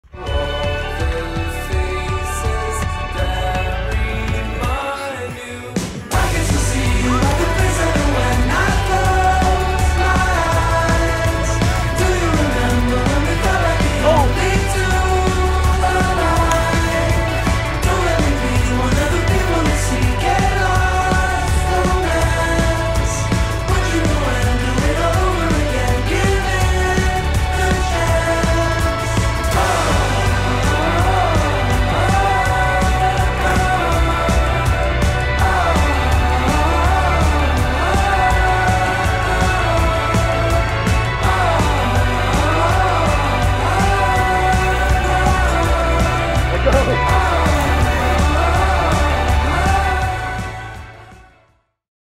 LAKE ROPESWINGING RECAP!!
spontaneous rope swinging lake trip on a 115° day :)

beware of spiraled barbwire!